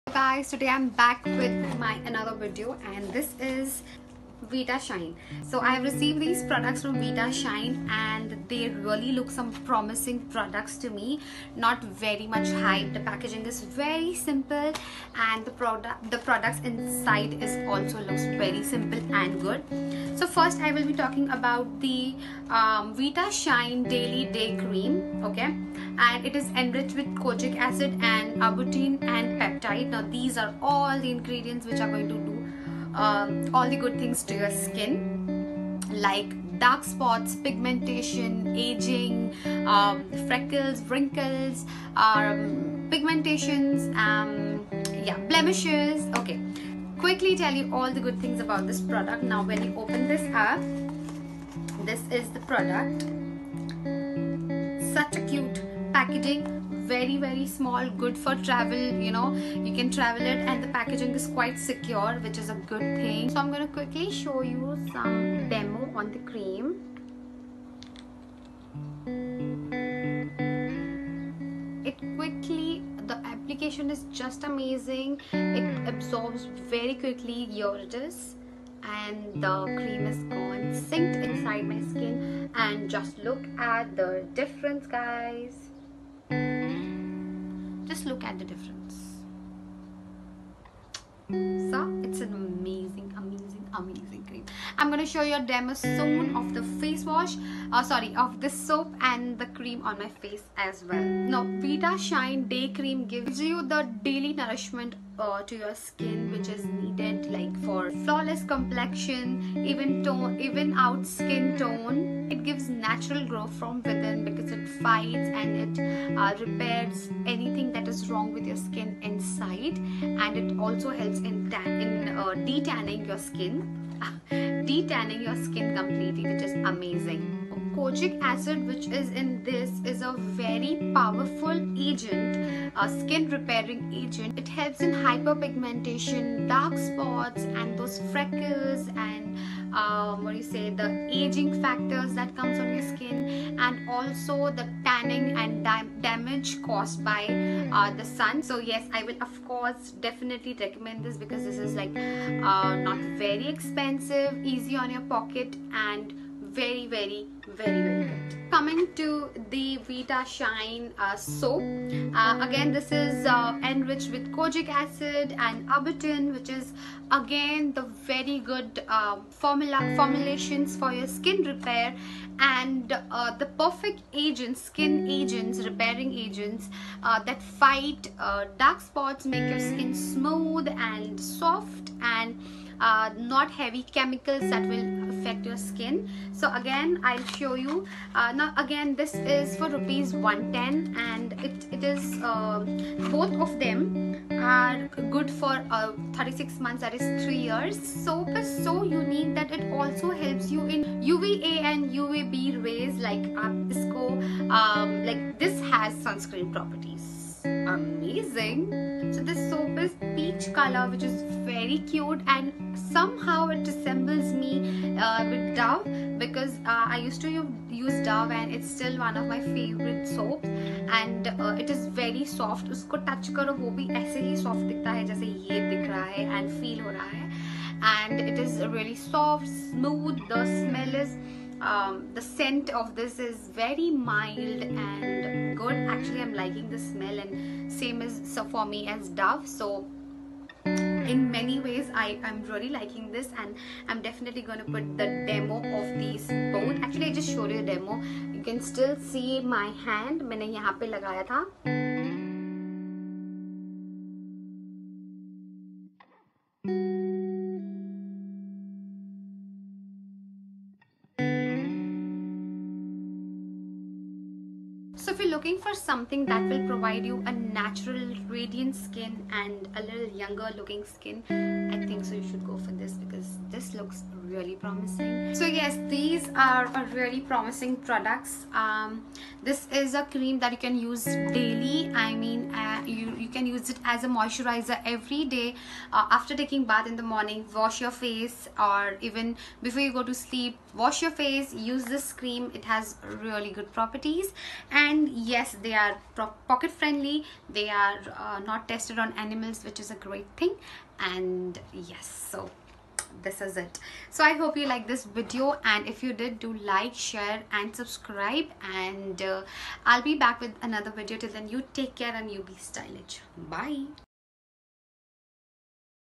No Scars, No Acne Marks with VITASHINE Daily Face Cream & Soap
Hello guys
Wel come back to my Channel.
Here's my  honest video of these amazing products from VITASHINE

Daily Face Cream

Face Soap